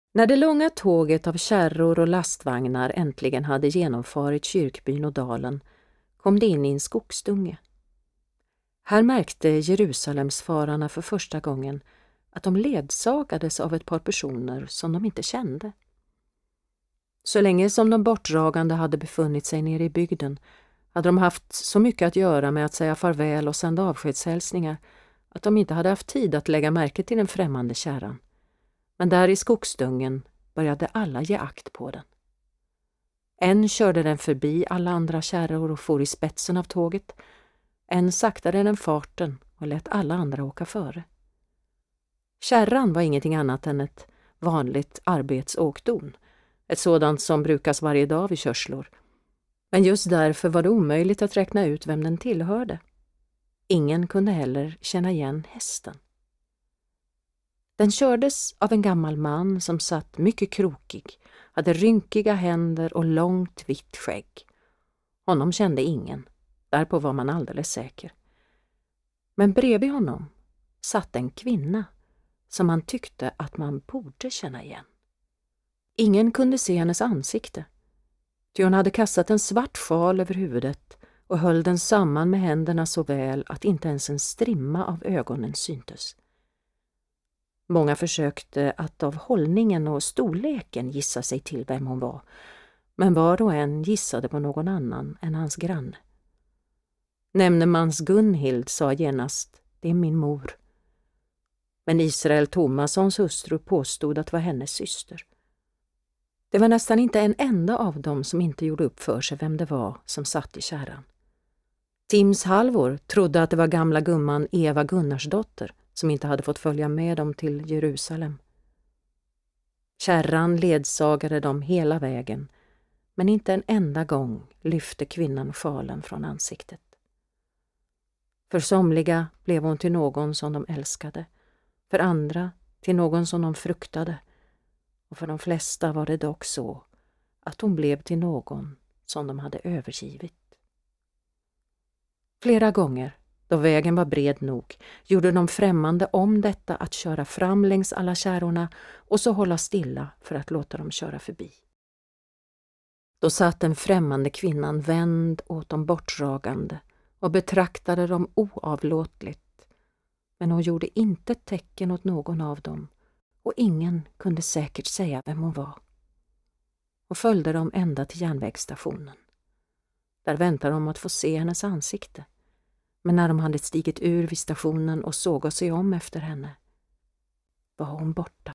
Gunilla Orvelius
Gunilla Orvelius läser ut Selma Lagerlöfs Jerusalem